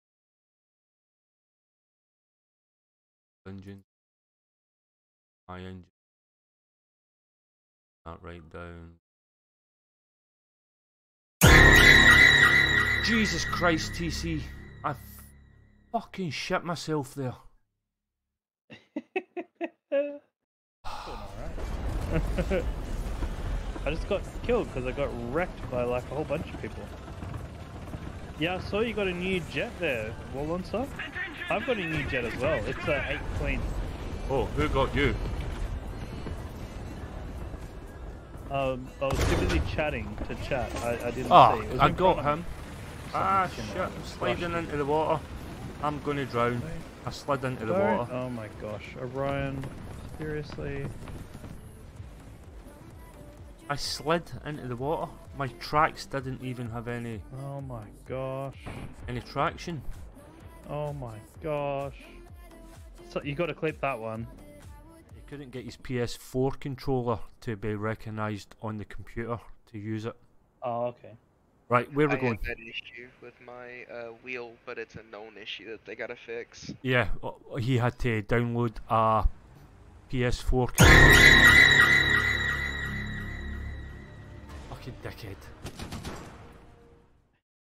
Tce trying to kill me
one of my regular viewers trying out my sound alerts